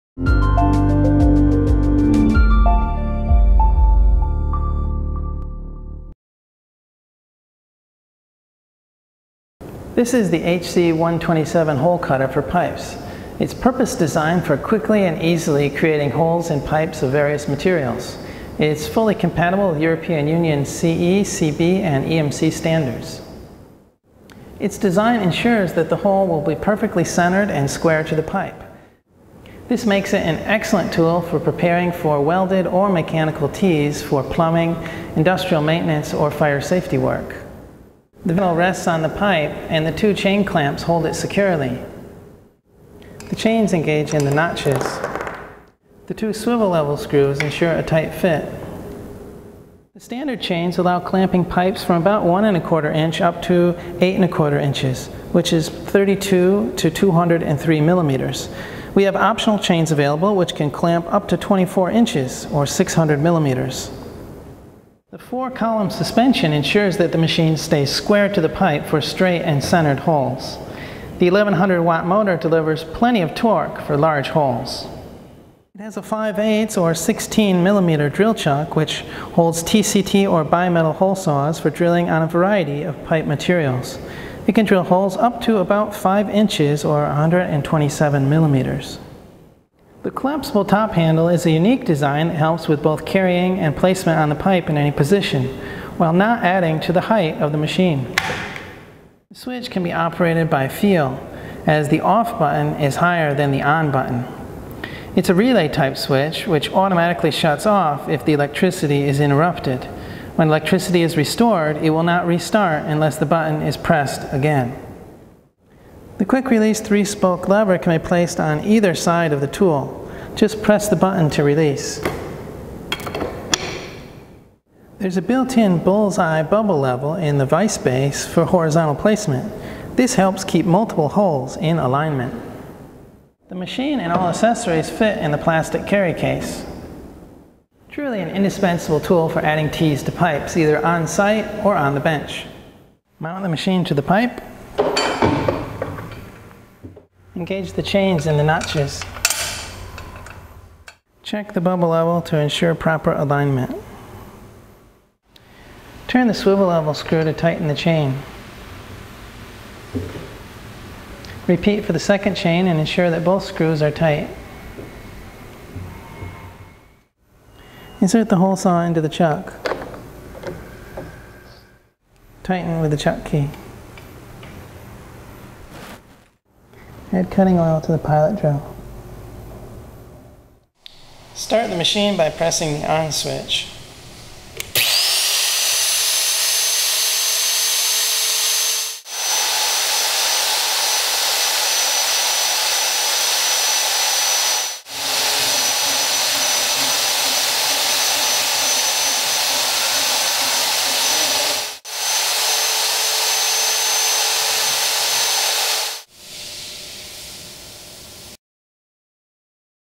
AGP【HC127 Hole Cutting Drill】Product Introduction and Operation
★ AGP【HC127 Hole Cutting Drill】
A convenient tool for quickly drilling perfectly centered and perpendicular holes up to 127 mm diameter in pipes from 1-1/4" to 8-1/4".

★ Product Information
Power Input｜ 1100W
Voltage｜
     ■ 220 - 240 V ~ 50 - 60 Hz
     ■ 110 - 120 V ~ 50 - 60 Hz
No Load min-1｜ 130
Max. Cutting Capacity (Hole Cutter)｜ 127 mm
Pipe Mounting Capacity｜ 32 mm – 203 mm (600 mm w/ optional Long Chain Set)
Drill Chuck Capacity｜ 1 mm - 16 mm
Over Load Protection｜ With
Net Weight｜ 14.5 kg ( 31.9 Lbs ) Not including Crank Handle